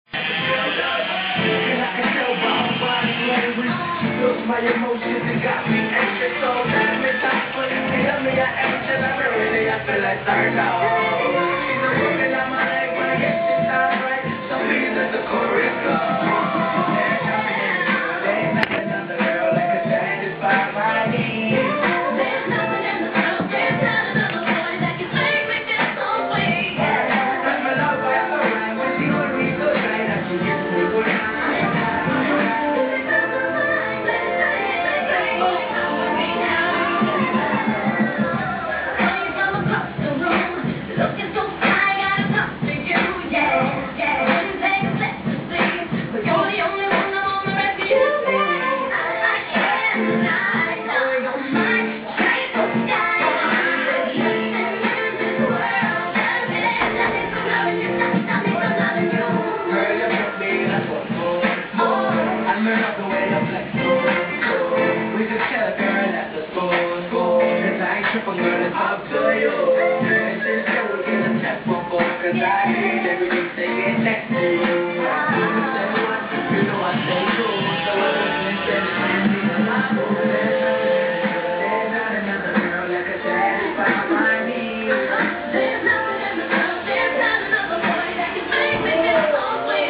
Logitech Z-5500 (505W) and Z-640(70W) by ingerasro !!
Acabo de recibir mis queridas Z-5500 !!!En este video se pueden ver tambien las Z-640 !!Q diferenciaa!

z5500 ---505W
z640 70W

:D:D:D